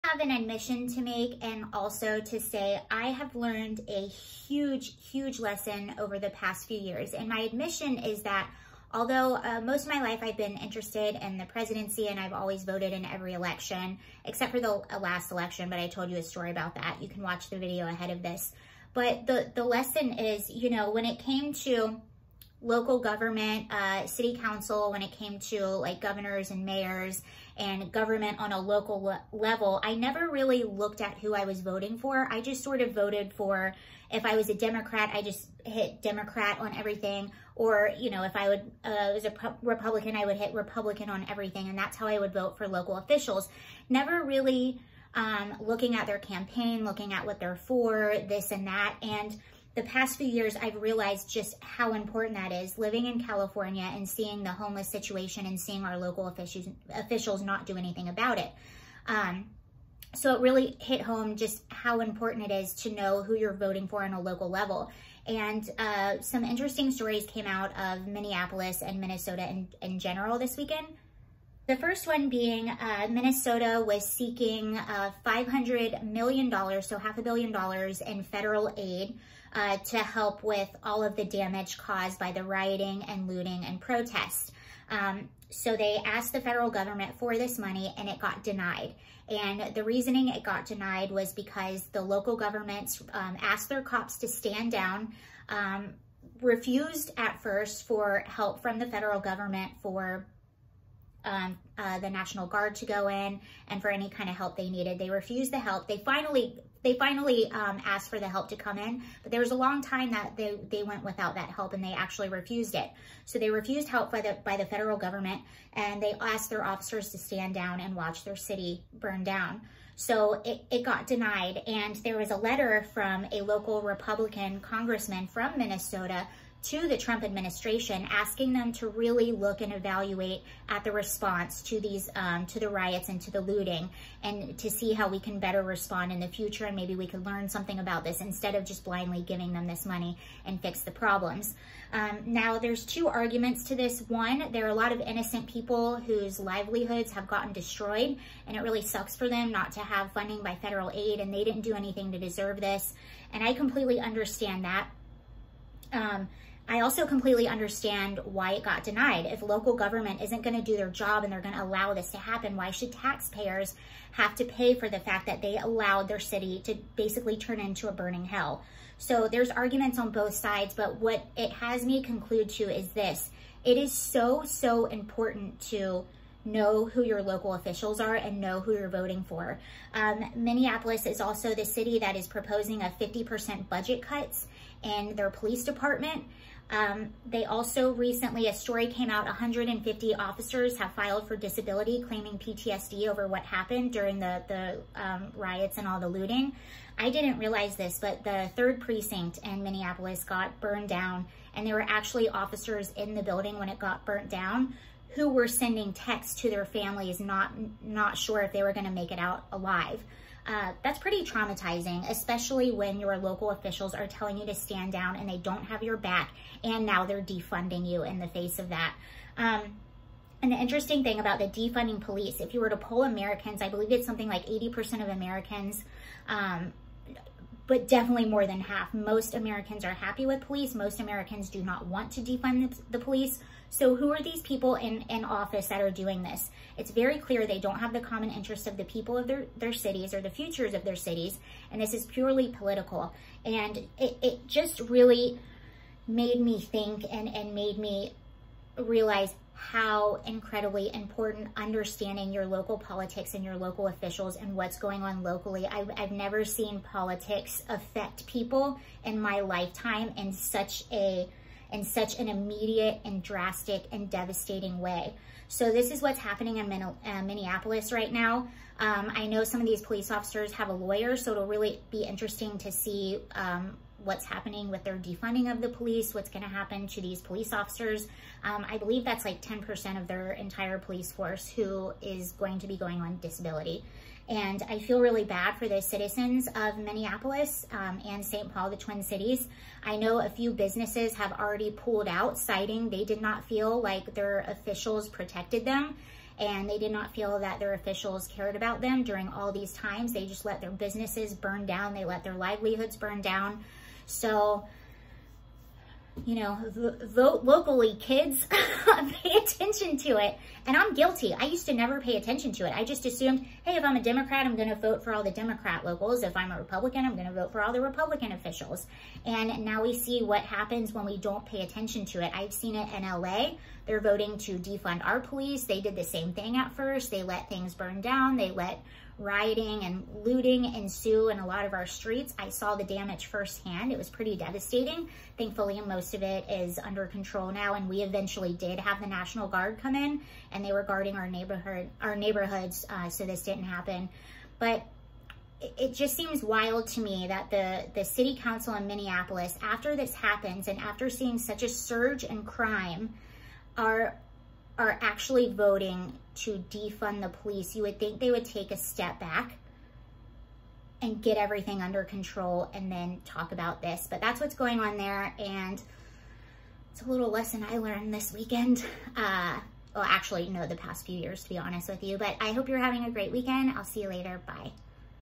500 Million Denied To Minnesota, 150 Minneapolis Cops Claim Disability
The federal government has just denied Minnesota aid due to all the damage caused in the rioting and looting. 150 police officers claim disability due to the PTSD cause by  the unrest over the past few months.